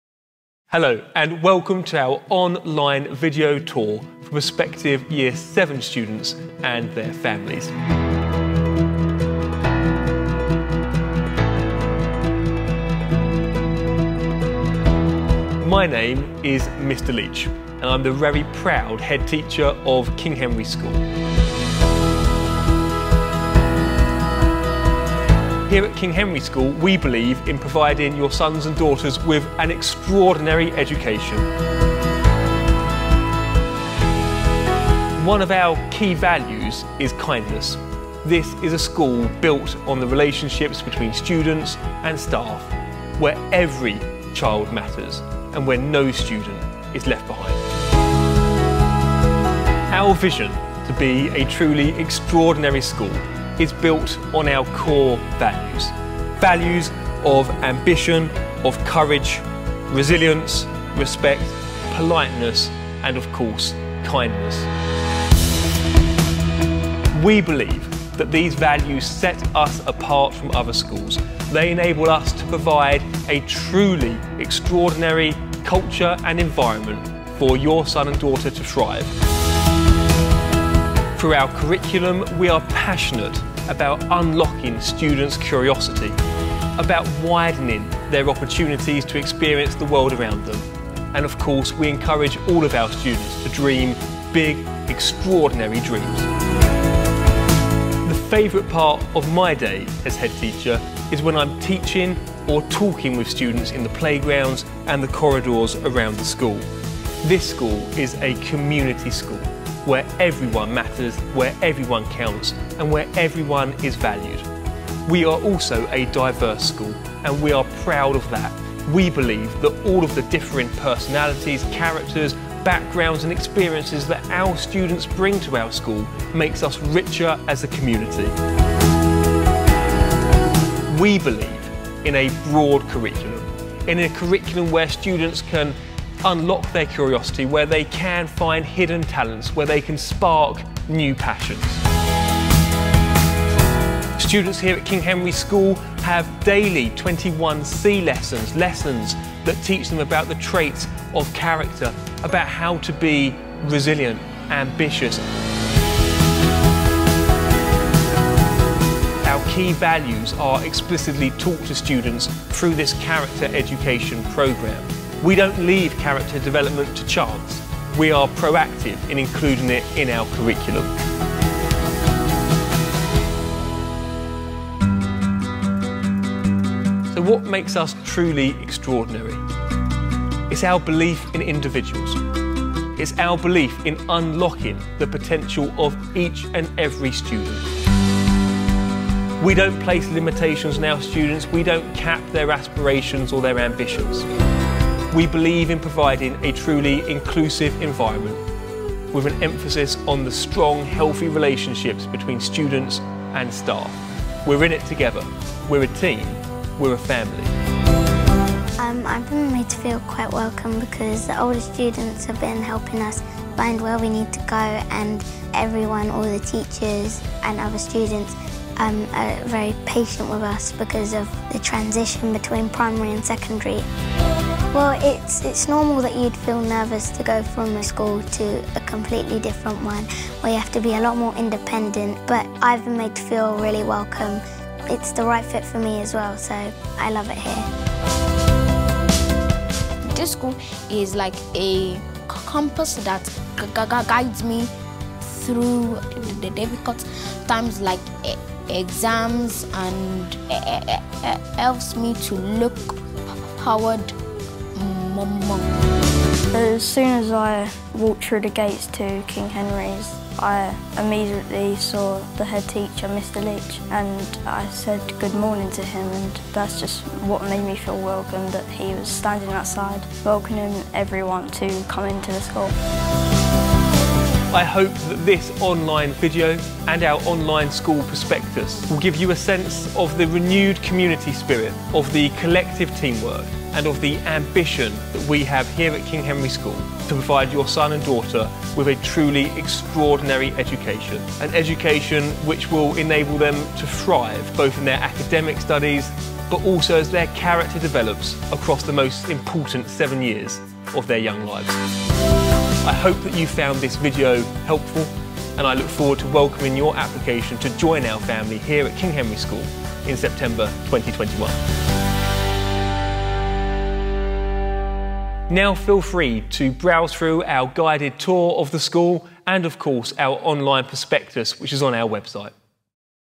King Henry School Welcome Video
We are delighted to launch our online King Henry School prospectus and video tour of the school. Despite not being able to run a traditional open evening, due to COVID-19, we are determined to ensure that every prospective Year 7 family is able to find out about our extraordinary school. We hope that you find these resources useful and we look forward to welcoming your application to join our KHS family in September 2021.

Mr R Leitch, Headteacher